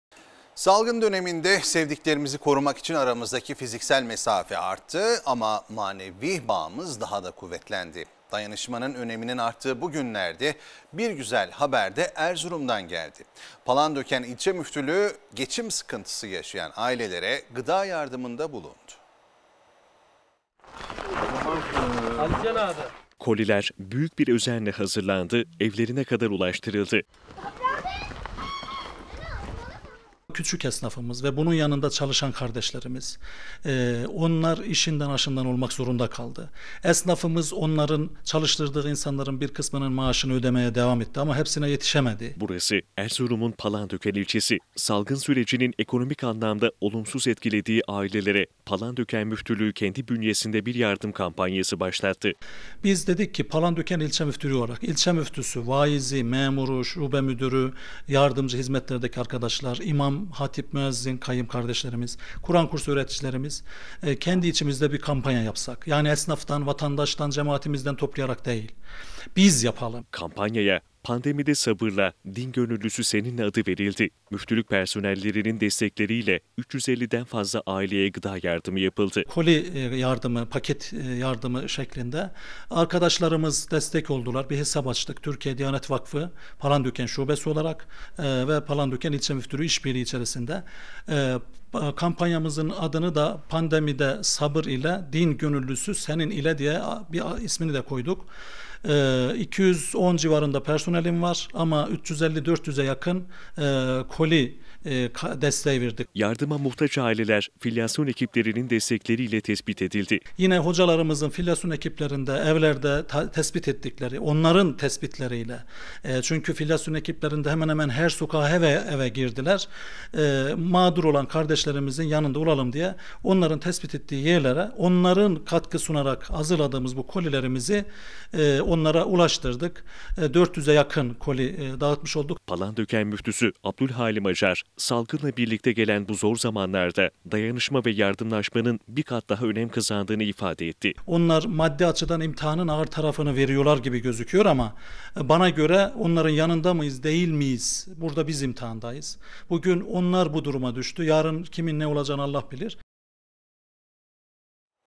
Palandöken Müftülüğü'nden Yardım Çalışması
Salgın döneminde sevdiklerimizi korumak için aramızdaki fiziksel mesafe arttı; ama manevi bağımız daha da kuvvetlendi. Dayanışmanın öneminin arttığı bu günlerde bir güzel haber de Erzurum’dan geldi. Palandöken İlçe Müftülüğü geçim sıkıntısı yaşayan ailelere gıda yarımında bulundu.